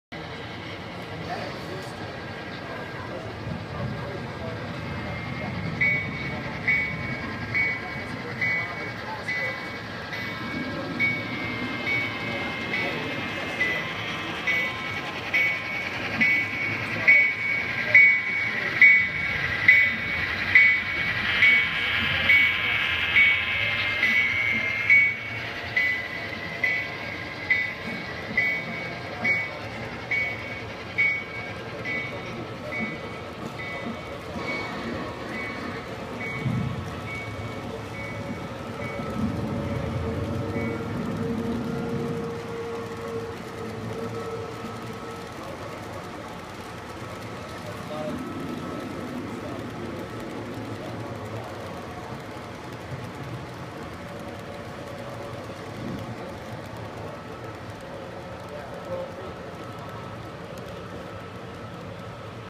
Genesis SCL units roll on The GSMRRC
My new Seaboard Coast Line Athearn Genesis  F units and a Intermountain B unit roll down hill on the Grand Strand Model Railroad Club at open house 12/30/2017. All with Tsunami sound